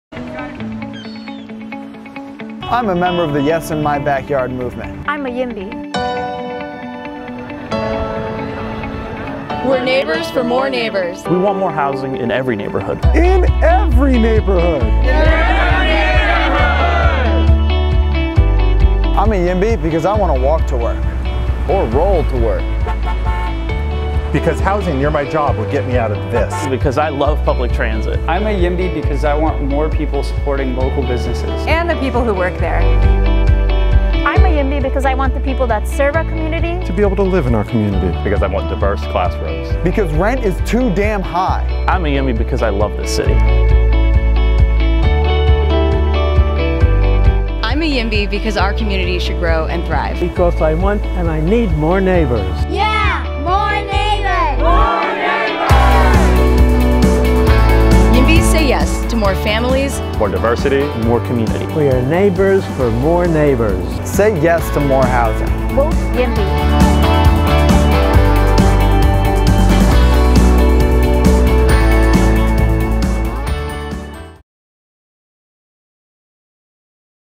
Vote YIMBY
Members of the Yes In My Back Yard movement talk about why they passionately pro-housing.